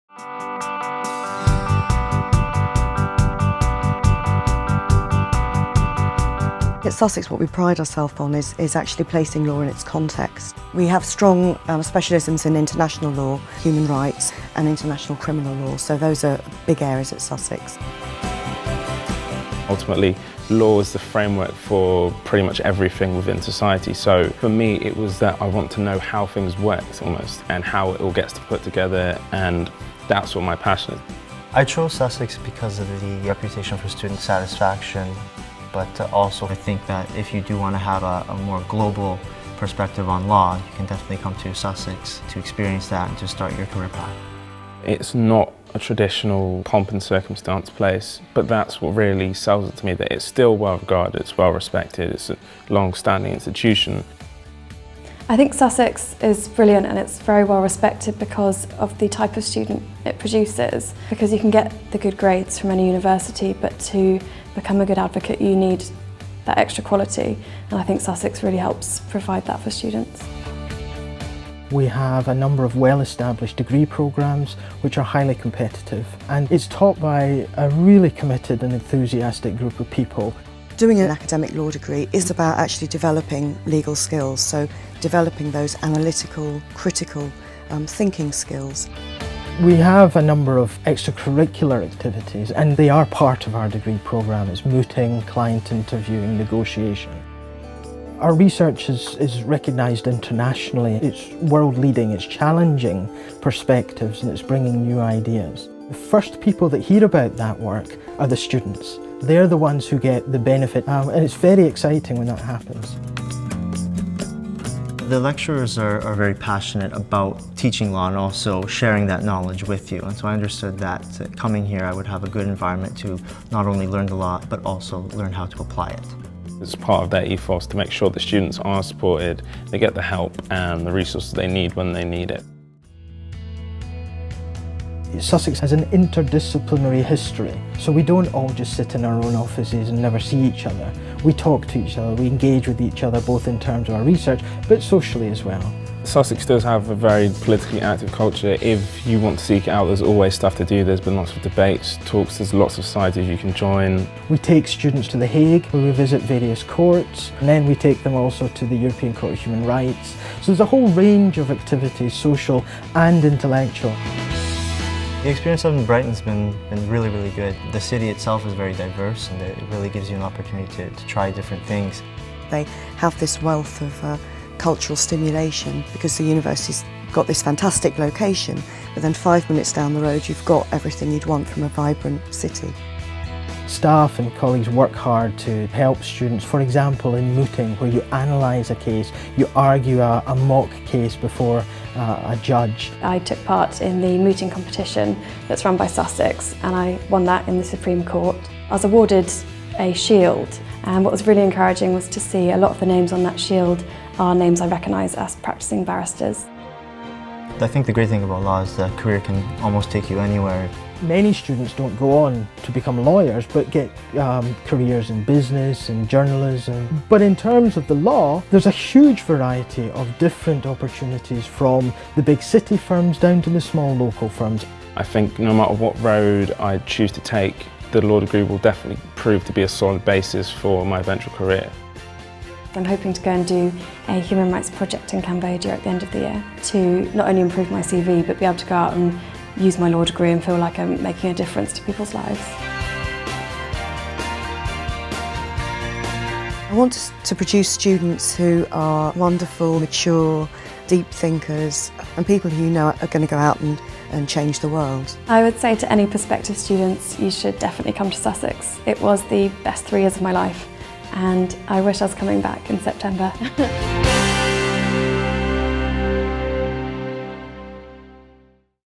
Studying Law at the University of Sussex
Staff and students talk about what it's like to study Law at the University of Sussex